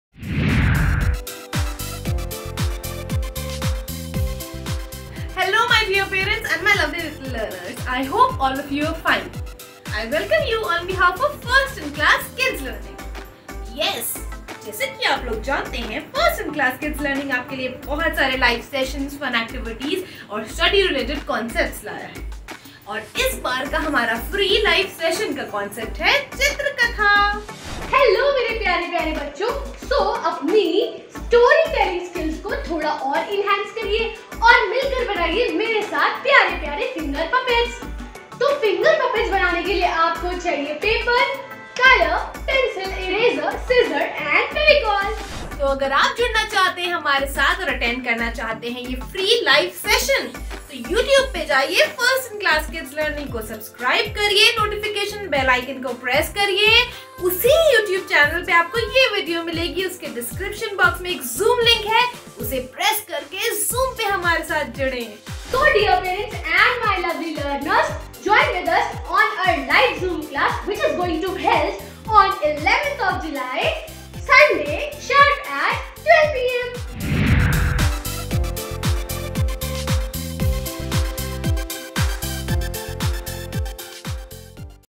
Free Live Class for kid’s |Kids activity & Pre-school education । First in class Kids learning
Hello! kids,
First in class organizing 17th free online class for kids on Sunday 11 July at 12.00pm.
In this class our expert teacher Iqra Salman will explain about Chitrakala. She will make you learn easy hand drawing techniques.
Our art and craft teacher Aayushi Verma will make you learn about finger puppet.
So kids be ready, install zoom app from play store and click the given below link to join free kids live class on our platform.

"First in Class is inviting you to a scheduled Zoom meeting.

Topic: First In Class Kids Learning - 17th Live Session at 12 Noon
Time: Jul 11, 2021 12:00 PM India